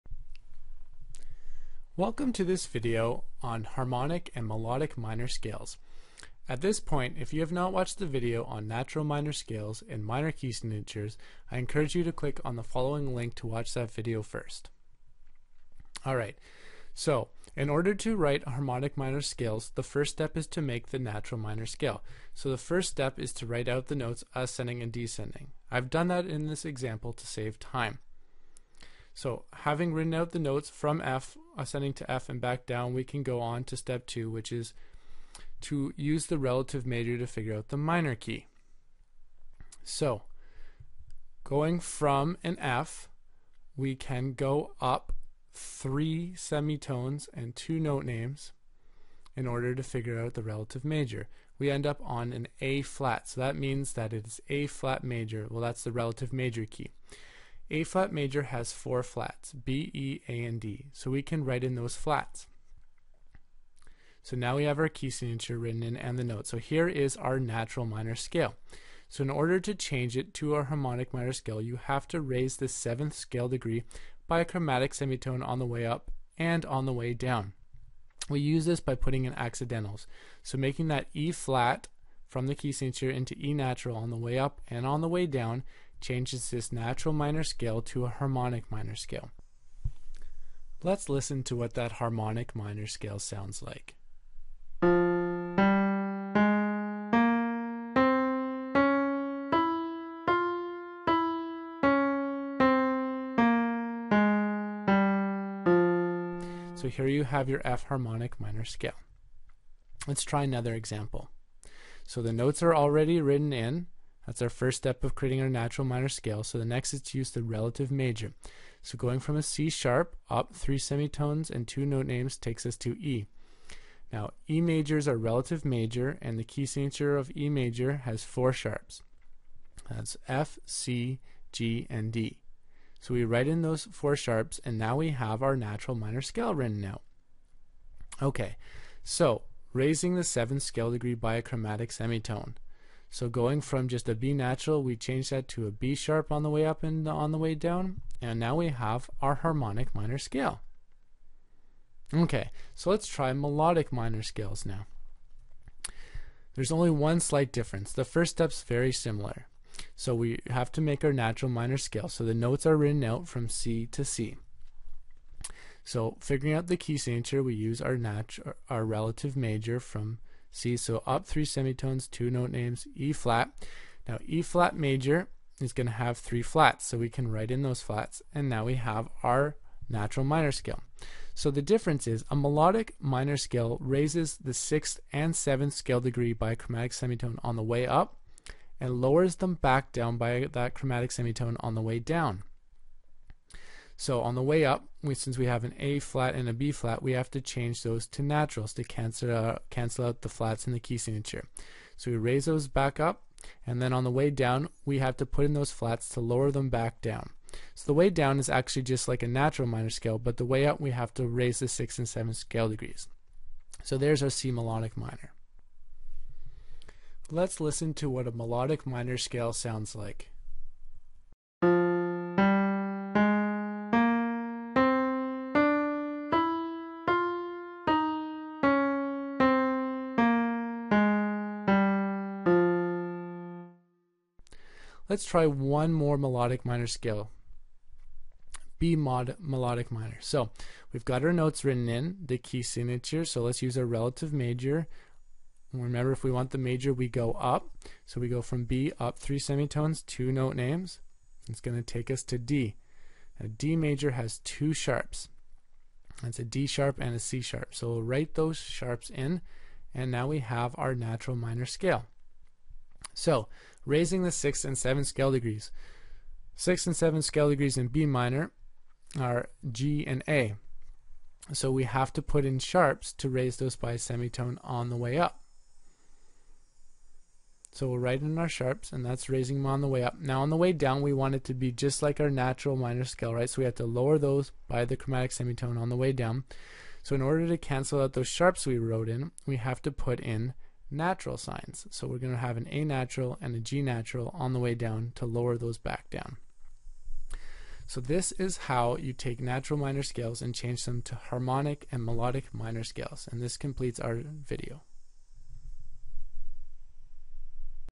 Harmonic and Melodic Minor Scales
This video explains harmonic and melodic minor scales.